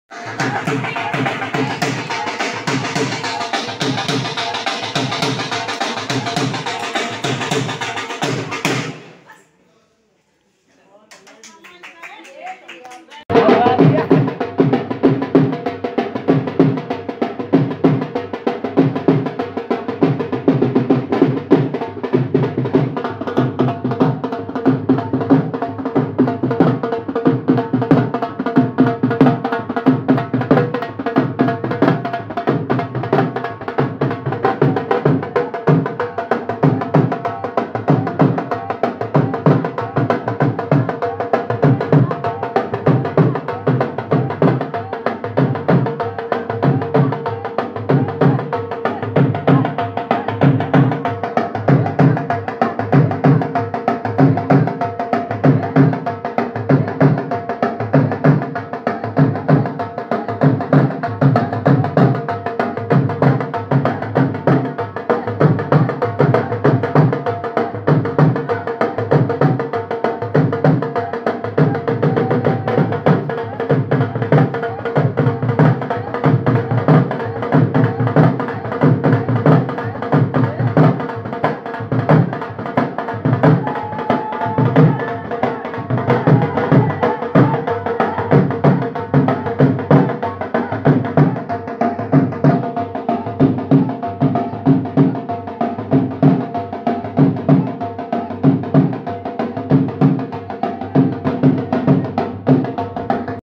ढोल की ताल से मिलाई ताल, कर दिया कमाल (५४१) राज रानी मल्हान आदिरास व कमलेश सिहाग
On The occasion of Holi celebration at Hisar in Jogi Ram Sihag's house
(M L A Haryana)